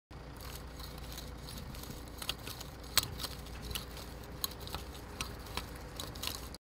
Fiskars QuikFit fűlevegőztető csillagkapa, gereblye fej
Kisebb gyepterület levegőztetésére használatos kapa, ahol kicsi a hely.
Ezáltal a tápanyag, műtrágya, magok, víz és levegő bekerülhet a földbe és könnyebben eléri a fű gyökerét. Lehetővé teszi a jobb tápanyagellátást.
Fűlevegőztető gereblyefej praktikus eszköz lehet a kerti munkák során a pázsit rendben tartásához. A makacsabb mohák és nádfélék eltávolításához is tökéletesen alkalmas. Önmagában vagy nyélhez csatlakoztatva is használható.